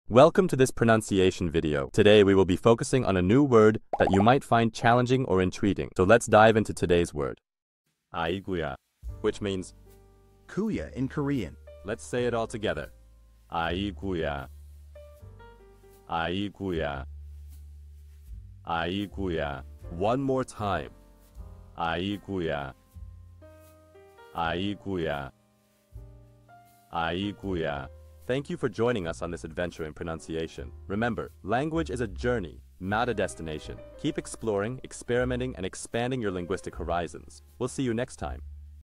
How to pronounce aiguya | 아이구야 (Kuya in Korean)
Master the Pronunciation of 'aiguya | 아이구야' - which means : Kuya in Korean 📝 with @PronunciationKorean 🎓🗣️ - [Your Guide to Korean]

Learn to pronounce 'aiguya' perfectly in Korean with our easy and engaging tutorial by @PronunciationKorean! 📖📽️ In this comprehensive video, we provide you with native speaker examples 🗣️👂 and phonetic breakdowns 🎵 to ensure you get the pronunciation just right.